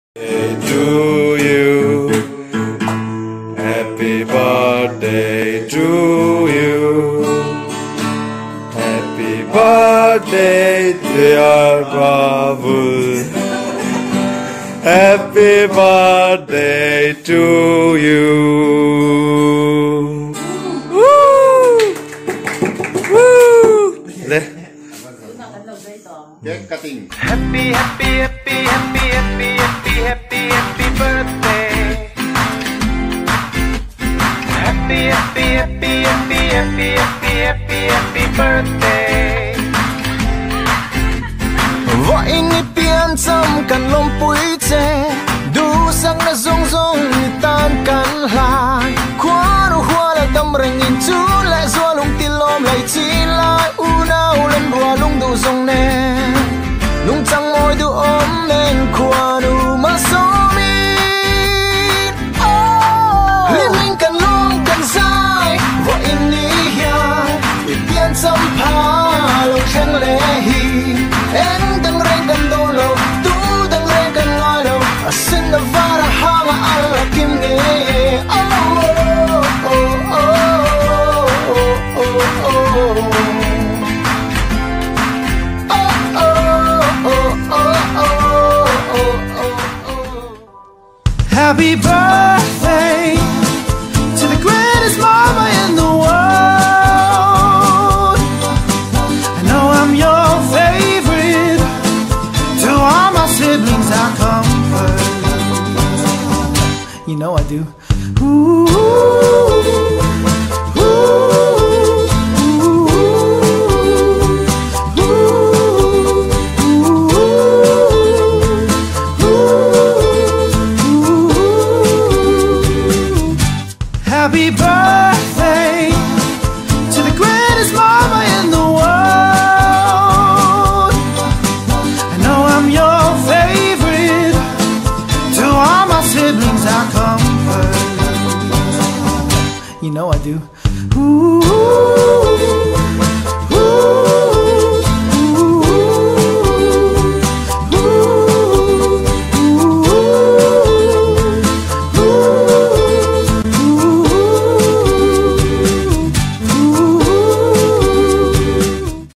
Ka Nu Birthday Lawmzan😊
Oct ni 27.2020...lockdown lai nia..hote in hlimtakin ka nu birthday kan hmang thei hi Pathian chungah lawmthu awm rawhse🙏